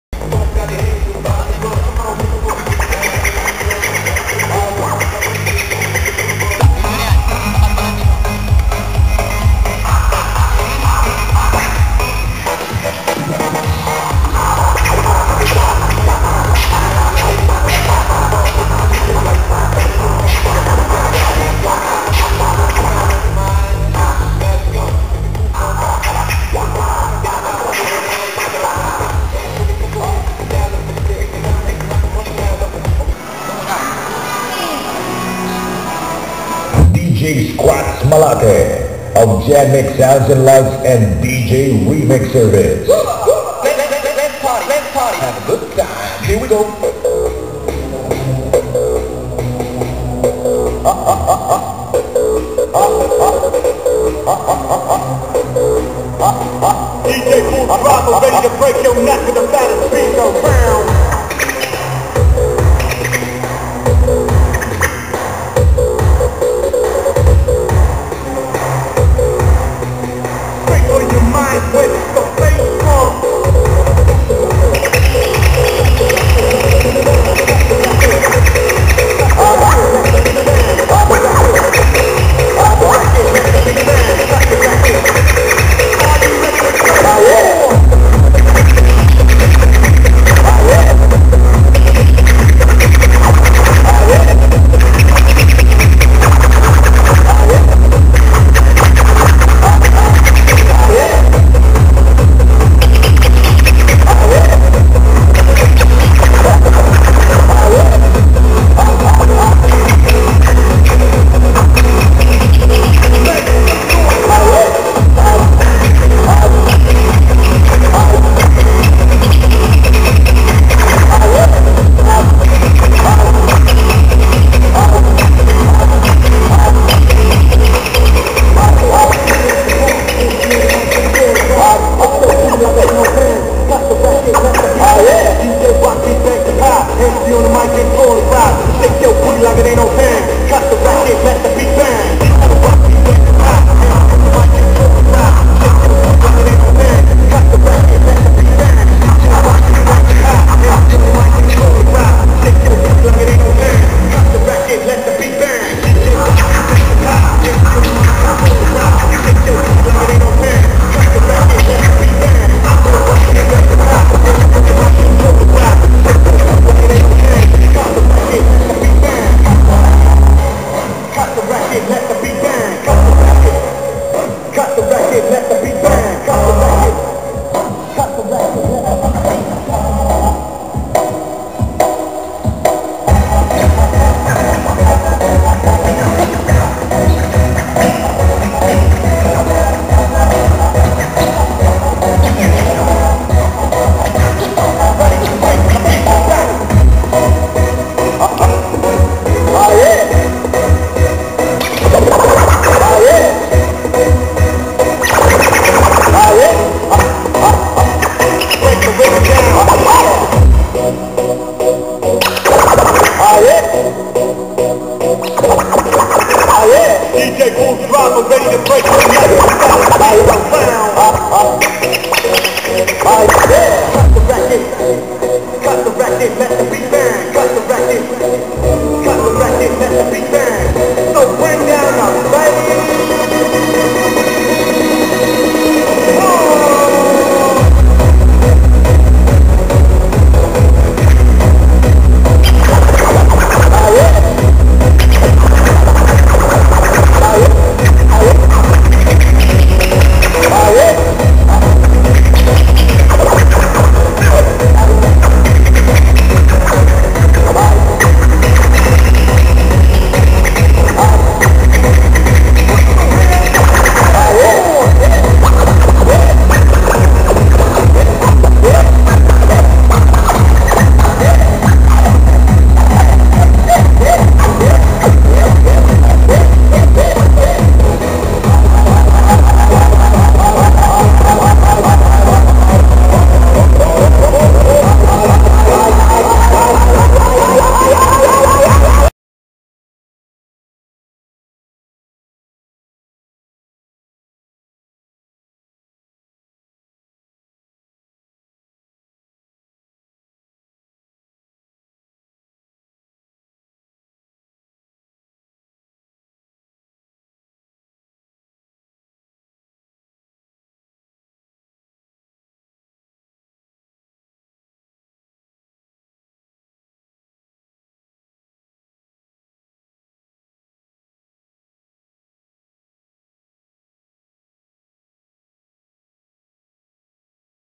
DJ SKRATZ OF JANMIX SOUND AND LIGHT @ CUENCA DAY SET UP
DJ SKRATZ OF JANMIX SOUND AND LIGHT MIXING LIVE @ CUENCA DAY CELEBRATION. NOV.03-07, 2015.